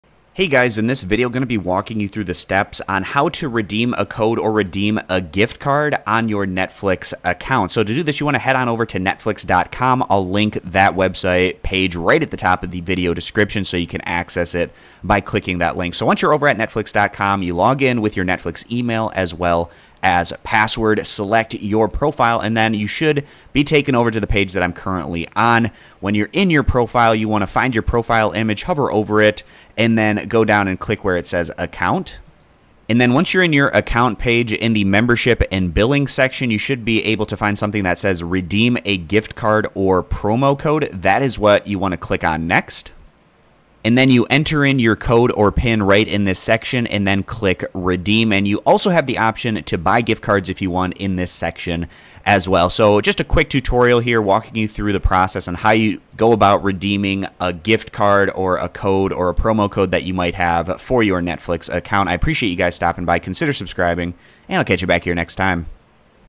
Netflix How To Redeem Code - Netflix Gift Card How To Use Redeem Instructions, Guide
Netflix How To Redeem Code - Netflix Gift Card How To Use Redeem Step by Step Instructions, Guide, Tutorial

In this video I walk through a simple and easy step by step instructions, guide, tutorial on how to redeem a Netflix Gift Card Code your Netflix profile account.